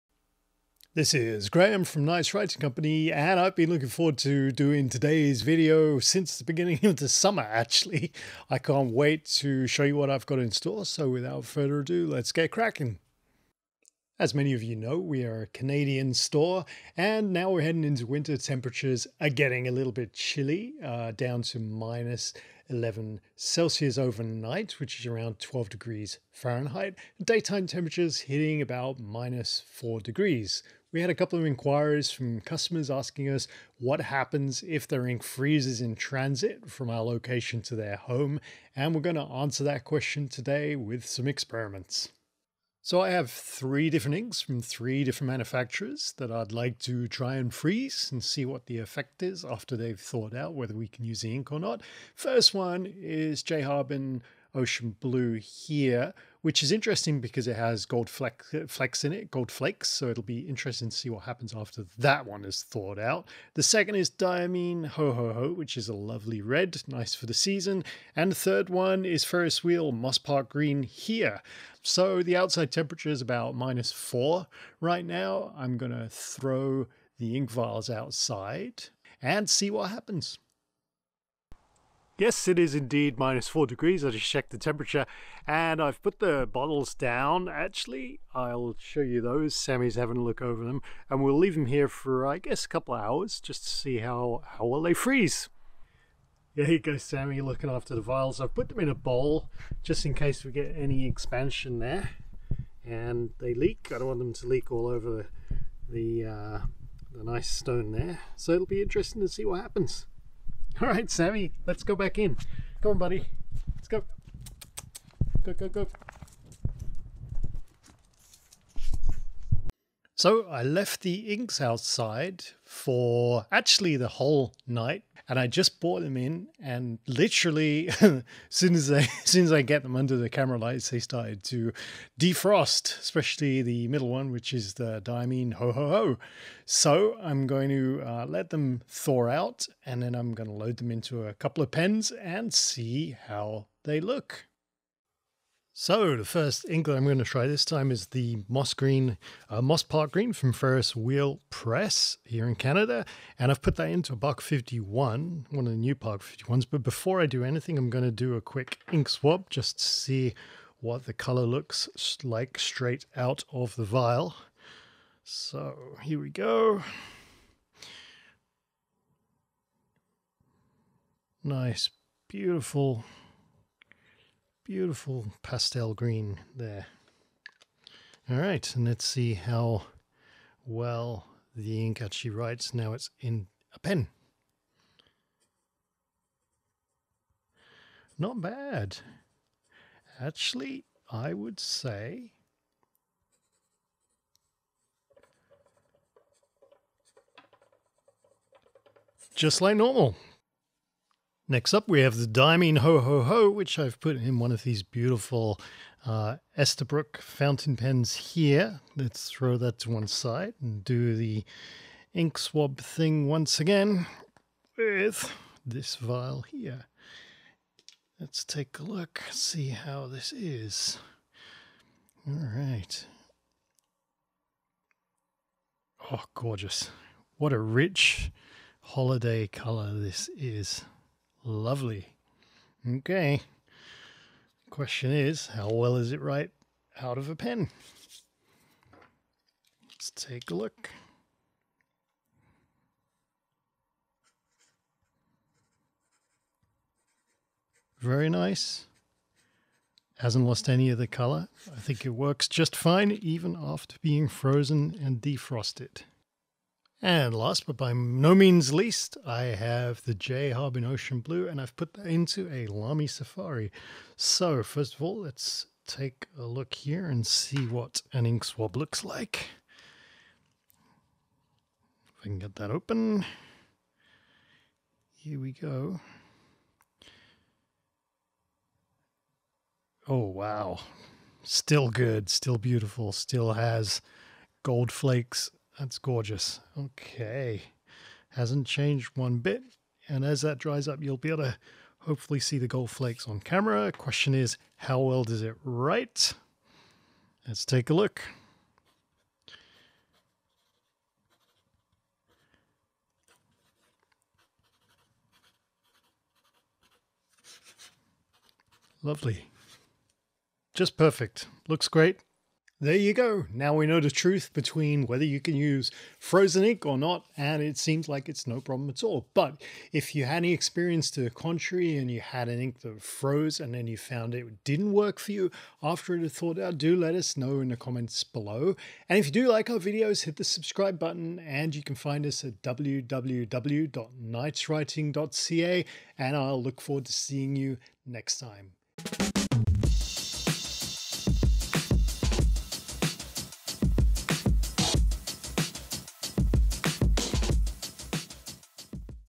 Can You Still Use Ink That Has Frozen?
In  video # 15 I take a look at whether you can use ink that has been frozen!

It doesn't happen often, but occasionally in freezes in transit to customers! Can you still use the ink after it has thawed out or do you have to bin it?

I do an experiment with three inks to find out the answer!

0:15 - It's Winter in Canada!
0:42 - The three inks I'm going to freeze.
1:21 - The ink vials go outside.
2:00 - The frozen ink vials.
2:25 - Defrosted Ferris Wheel Press Moss Park Green.
3:21 - Defrosted Diamine Ho Ho Ho.
4:16 - Defrosted J. Herbin Ocean Blue.
5:12 - So can you still use ink that has frozen?